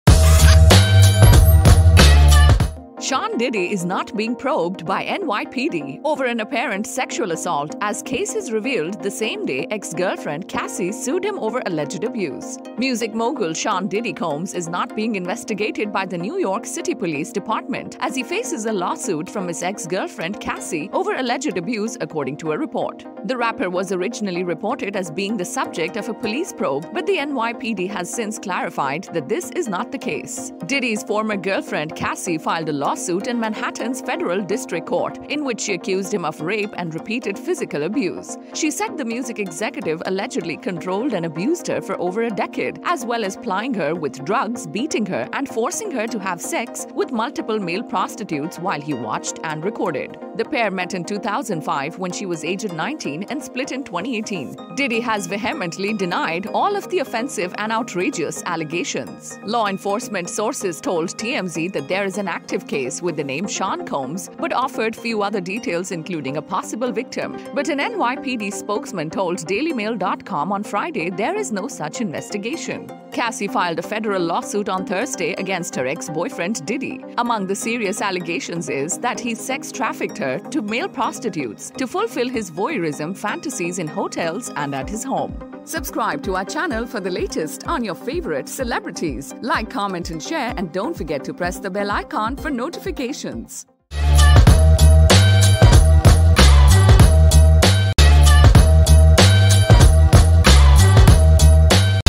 Diddy faces lawsuit from ex-girlfriend over alleged abuse - The Celeb Post
Sean 'Daddy' Combs is currently under investigation by the New York City Police Department as he faces a lawsuit from his ex-girlfriend Cassie, who has accused him of alleged abuse.

Sean Combs | Diddy | NYPD Investigation | Sexual Assault Allegations | Cassie Lawsuit | Celebrity Legal Issues | Entertainment News | Celebrity Scandals | Lawsuit Updates | Assault Accusations